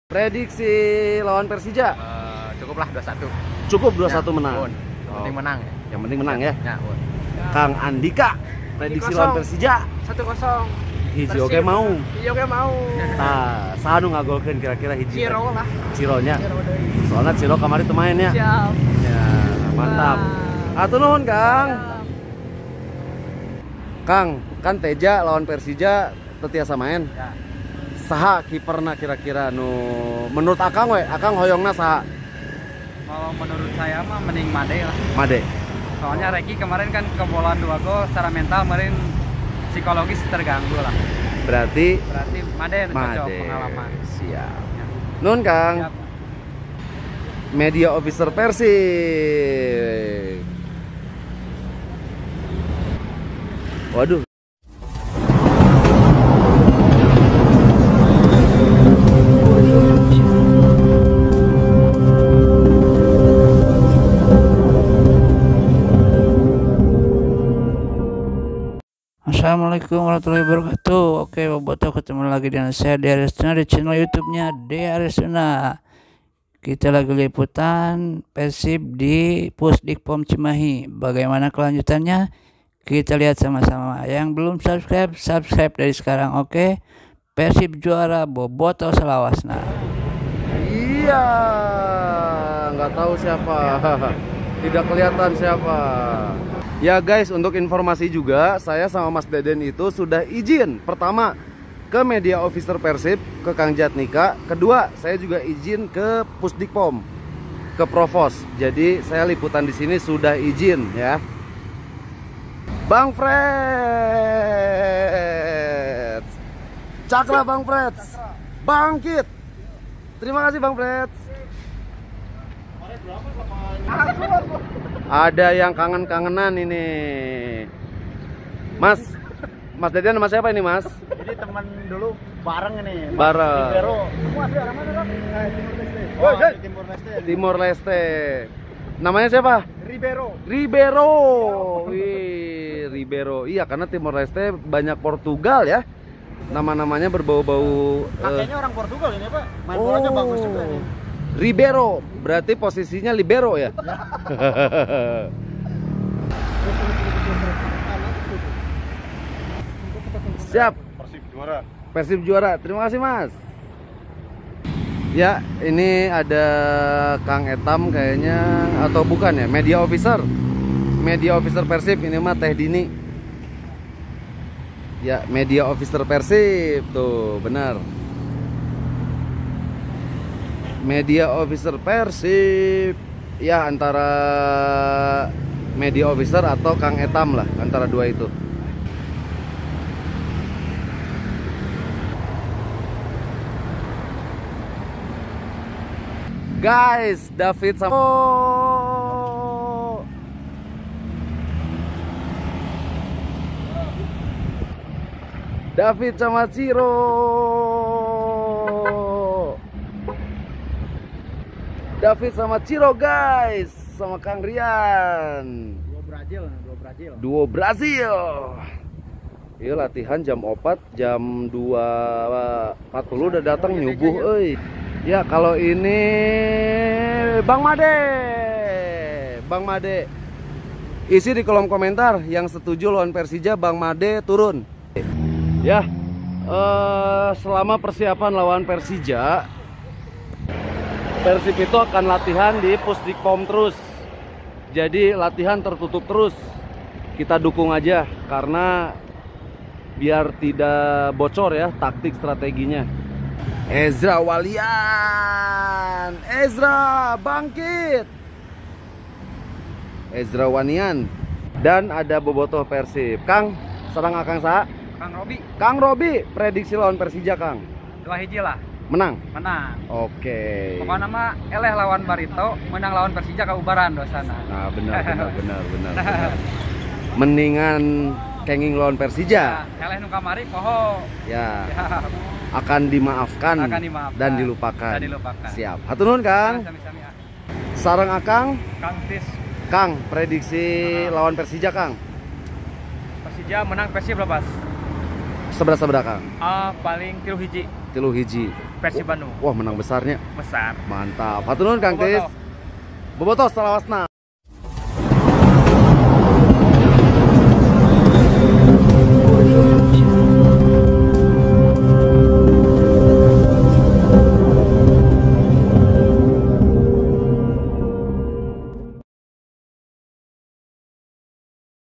Sisi Lain Kedatangan Para Pemain Persib Latihan Di Pusdikpom Cimahi|Tertup
Sisi Lain Kedatangan Para Pemain Persib Latihan Di Pusdikpom Cimahi|Tertup Meskipun Latihan Persib Sekarang Ini Akan Lebih Sering Tertutup Tapi Kita Dan Yang Lainnya Sebagai Bobotoh Akan Seelalu Mensupport Para Pemain Persib Khususnya Walaupun Hanya Lewat Jendela Mobil Mereka.
source video by:@gigalauda
ijin:aprovied